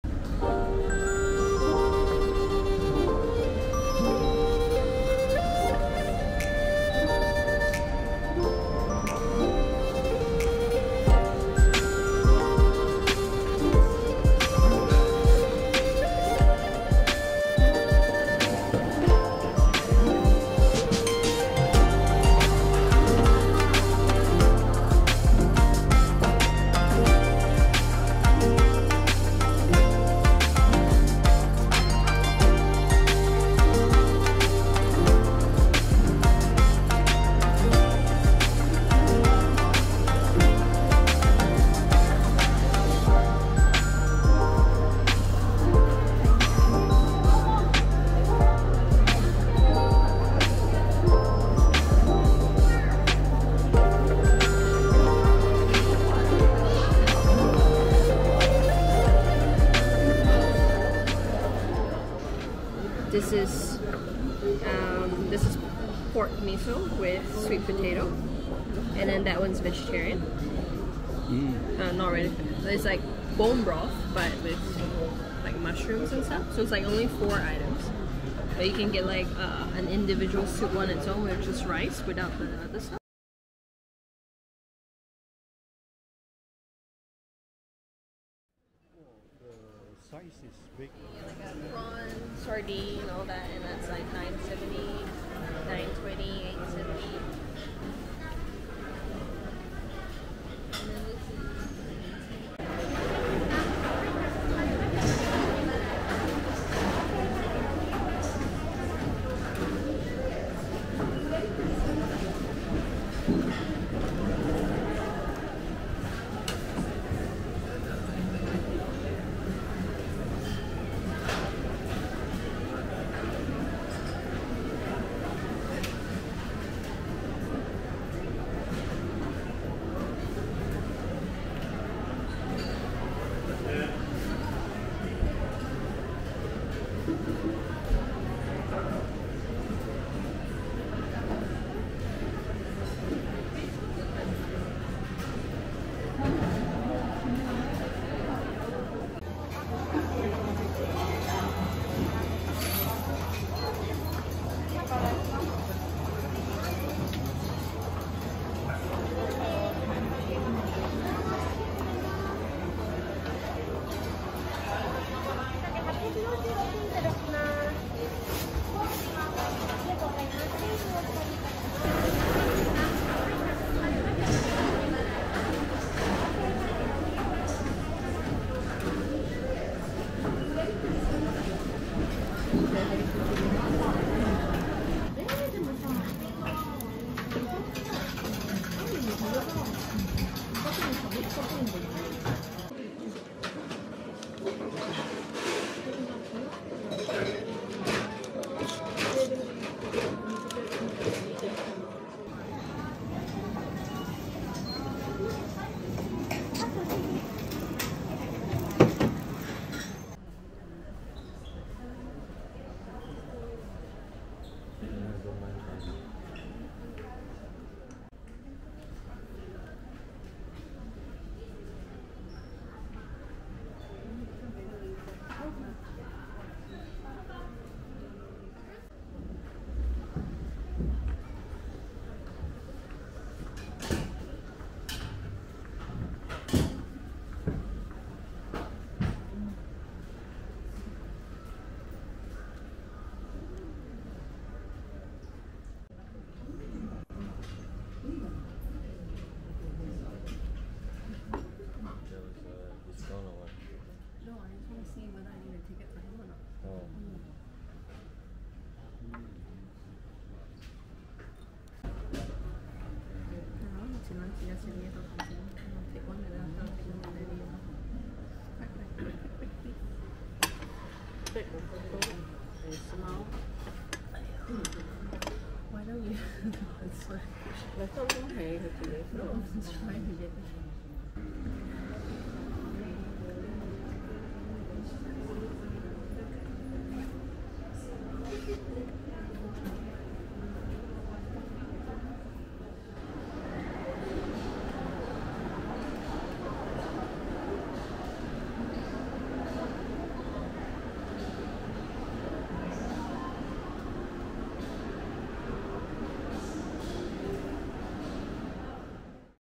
Tokyo Midtown Akasaka, Minato City Tokyo Japan - Delightful Restaurants Cuisine & Food-centric Shops
Tokyo Midtown is known for its fashion and design stores, but it’s also home to delightful cuisines and food-centric shops.